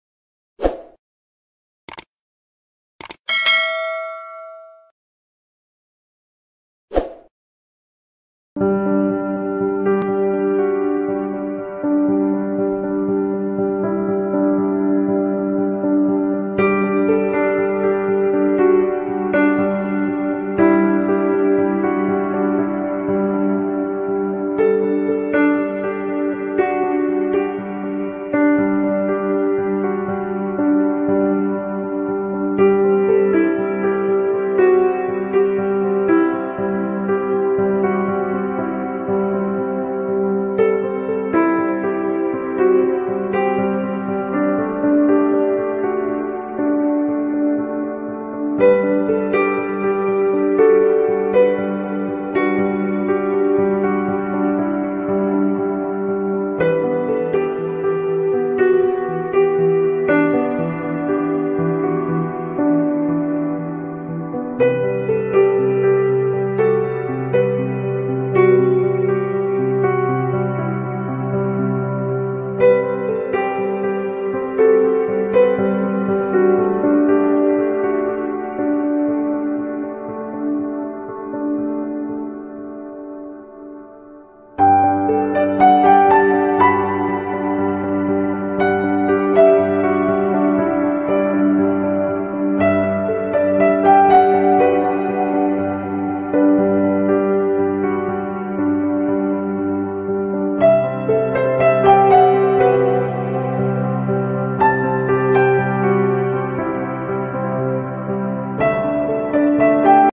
cng cylinder Tray Stand సిలిండర్ కి స్టాండ్ చేయడం జరిగింది
Material cost Rs:350/- current bill: 100/- In outside they asked 1500/- to 2000/-